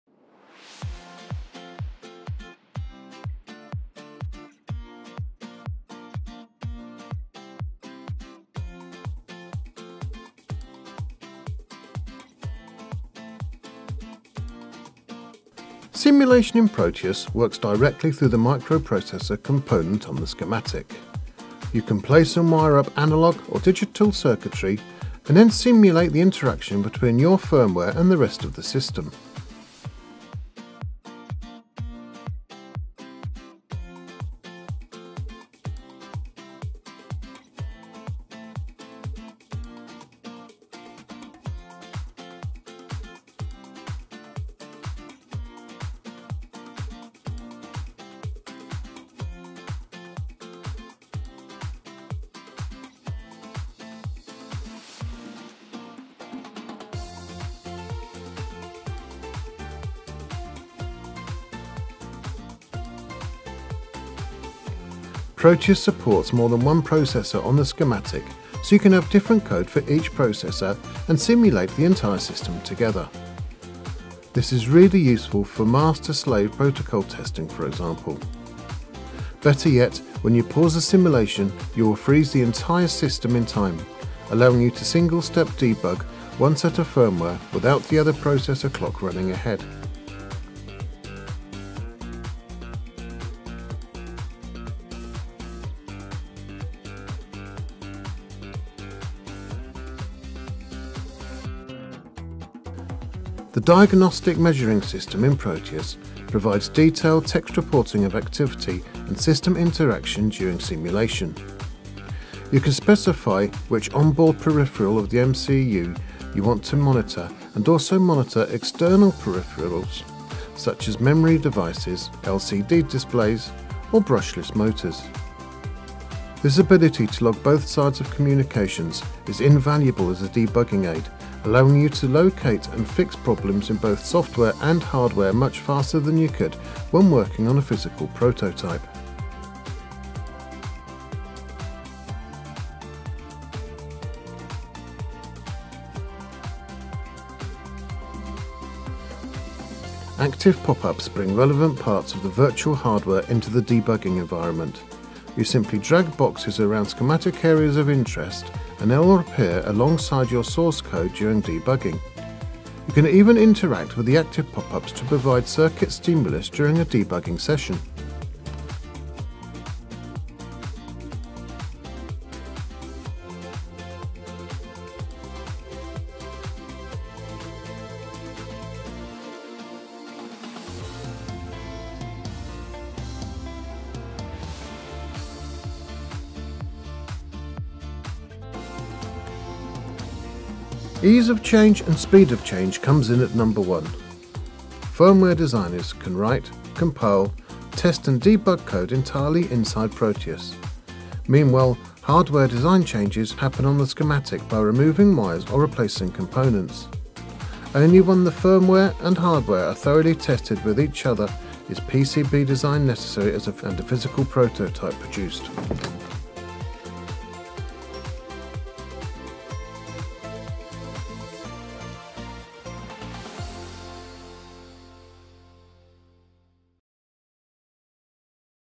Proteus VSM Top 5 Features
A highlight of the Top 5 features found in Proteus VSM for microcontroller and schematic design simulation.